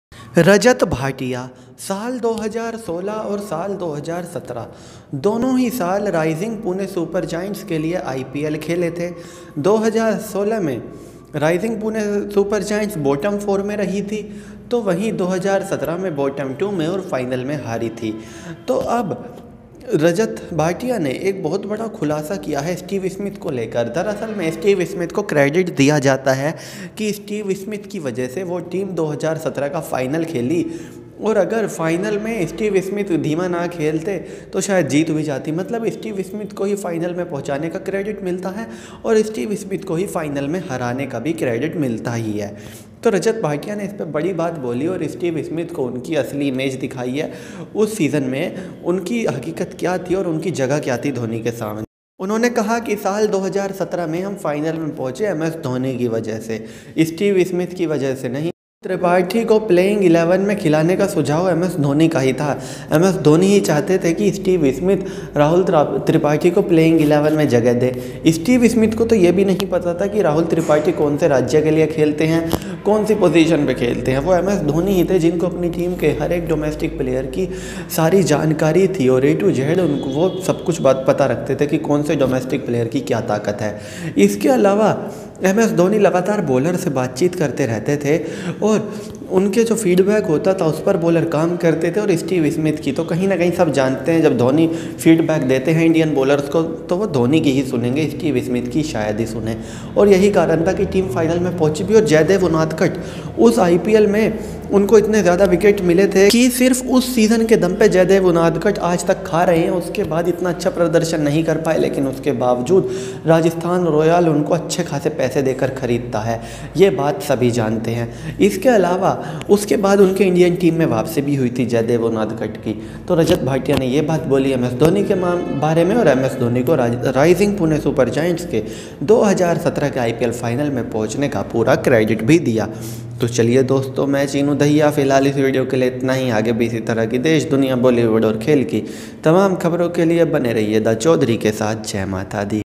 Rajat Bhatia ने Steve Smith के बारे में ऐसा क्यों कहा | Rajat Bhatia on steve smith latest
rajat Bhatia gives credit to ms dhoni for 2017 rising pine supergiant ipl success.